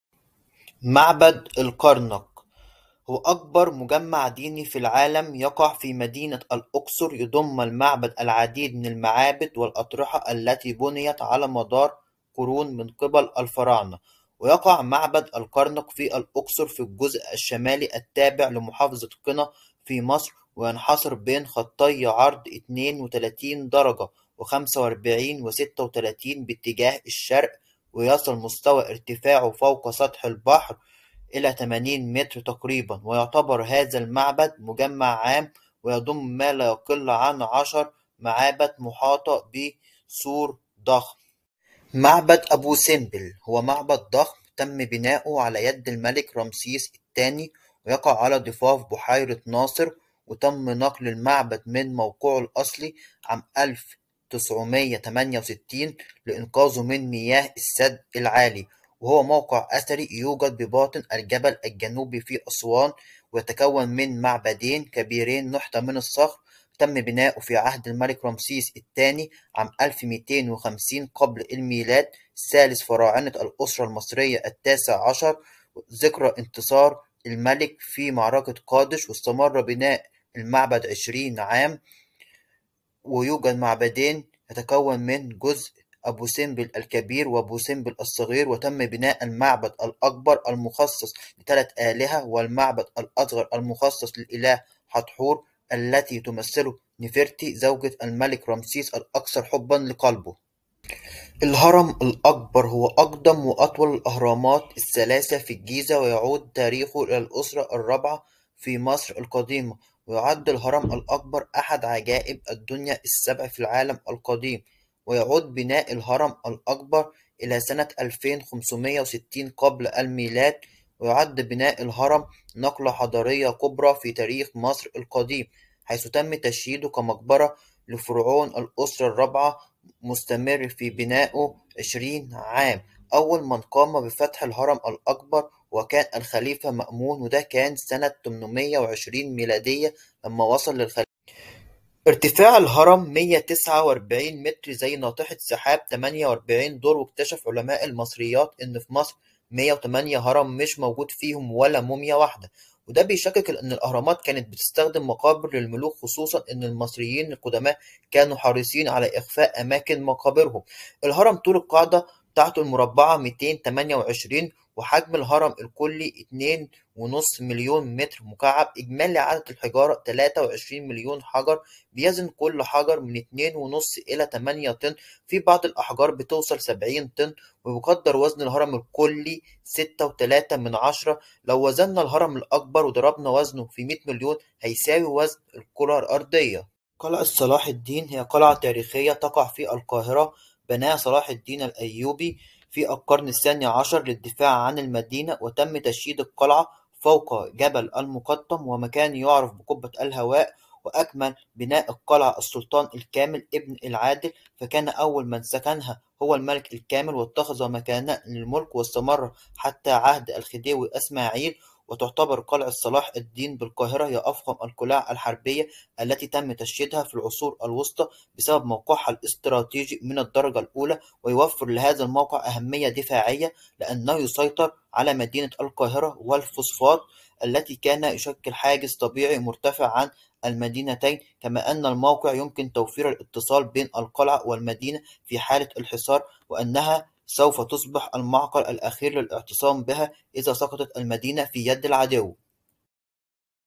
افضل الاماكن السياحيه في مصر
سنتحدث في هذا الفيديو عن افضل الاماكن الاثريه والسياحة في مصر مثل المعابد الاهرامات